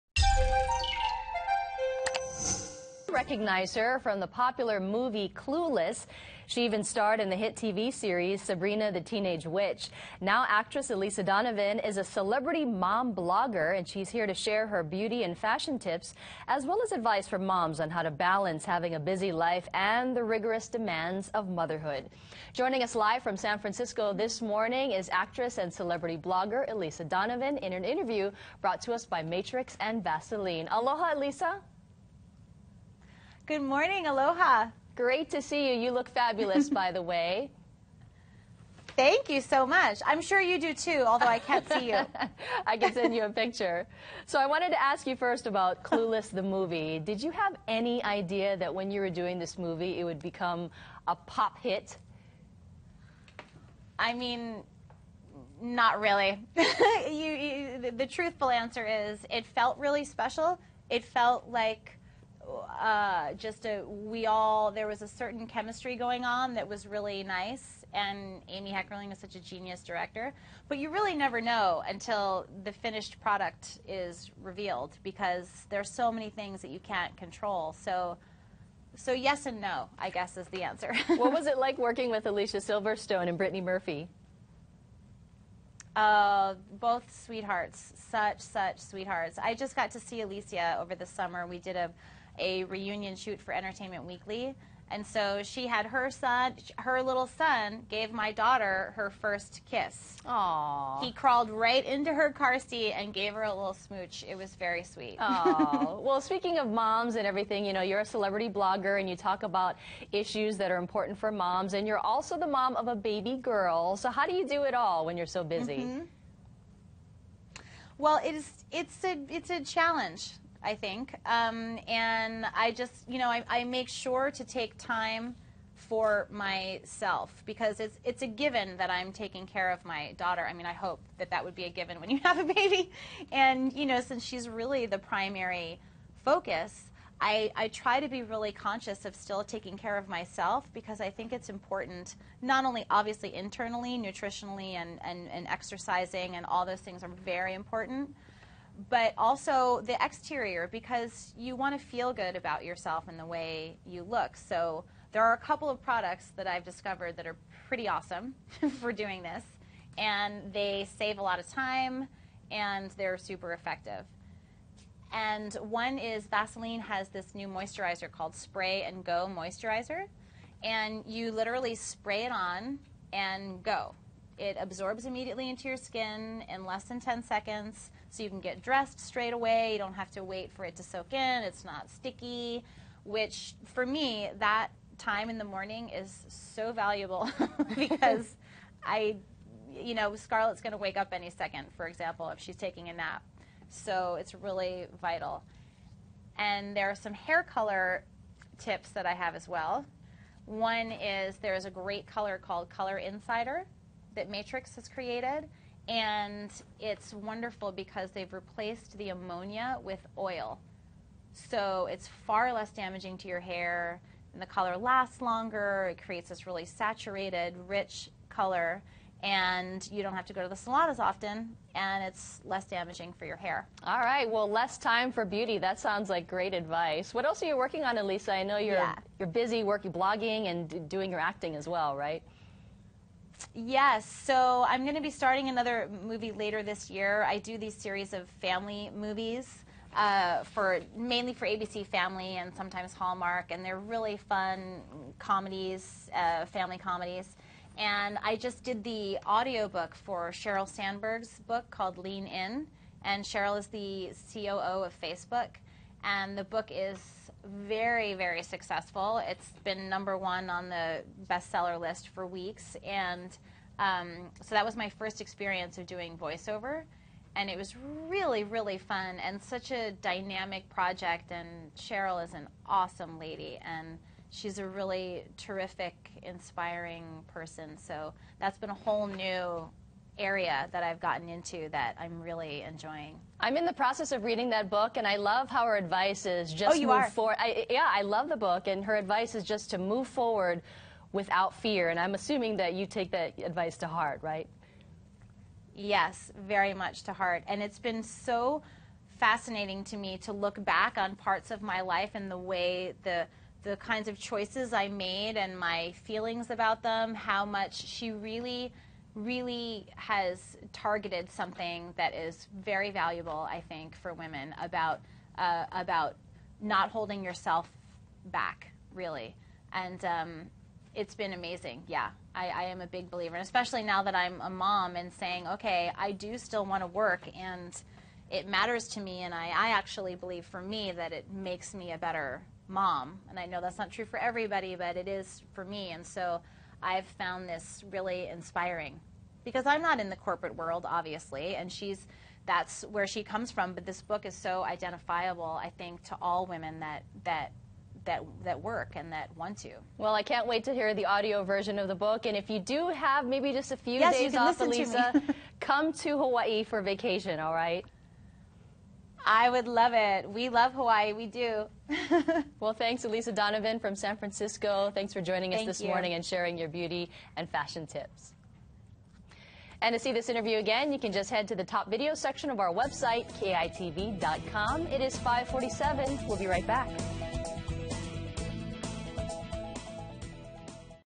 Elisa Donovan shares her tips for working moms
You may recognize her from the popular movie Clueless. Now actress Elisa Donovan is a celebrity mom blogger, and she came on KITV4 Morning News to share her beauty, fashion tips, and advice for moms balancing a busy life. For news you need to know,